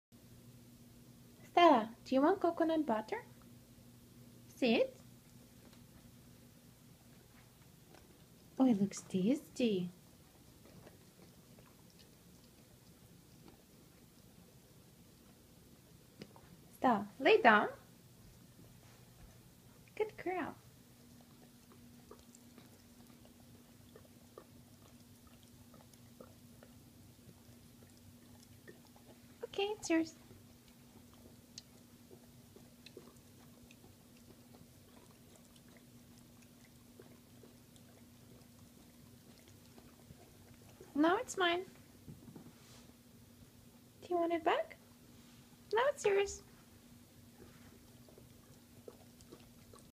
Stella the Doberman gets a Kong with Coconut butter !
Coconut butter is an amazing antimicrobial, anti fungal and antibacterial agent that will also make skin n fur very healthy! Also practice the give and take away to make sure ur dog doesn't get food aggressive. Don't remove it quickly instead remove toy:good slowly with confidence.